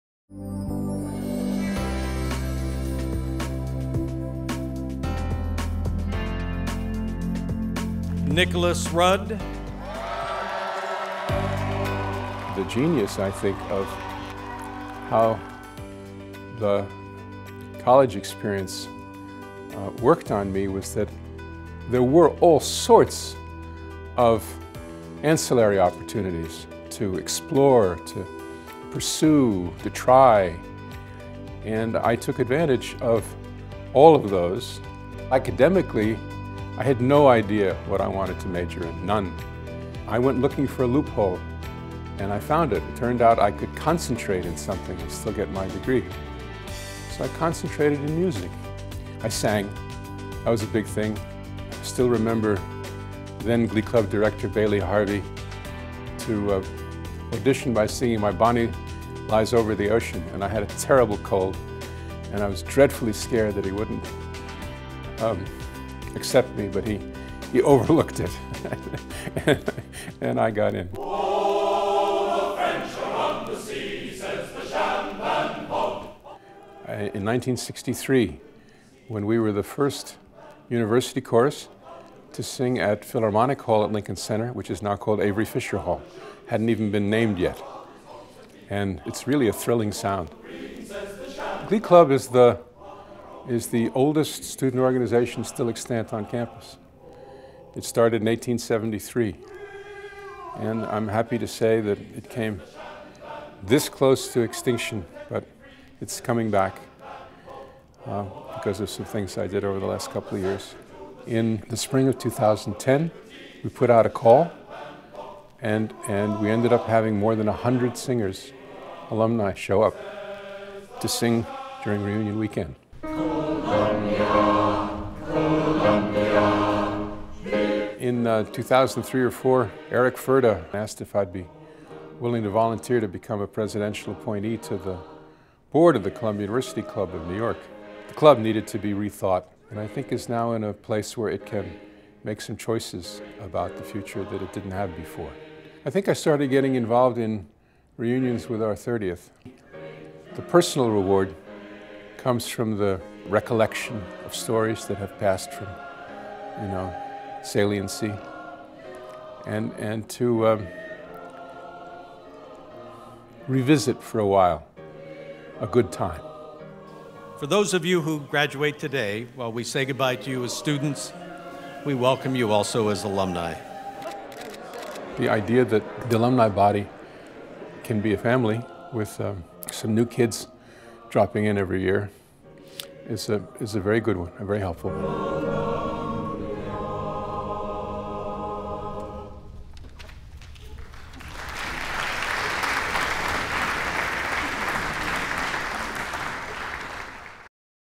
2012 Columbia Alumni Medalist Nicholas Rudd
Nicholas Rudd '64CC, '67BUS passed away on September 14, 2012. He was the prime mover in reviving the Columbia University Glee Club, the oldest extant student organization on campus, and in creating Columbia Alumni Singers, the affiliation group that grew from that revival, producing alumni sings at the College's Alumni Reunion Weekend and gathering historical information for the University Archives. A business consultant, Rudd began co-chairing his College class's reunions with its 30th and at the time of his death was working on its 50th.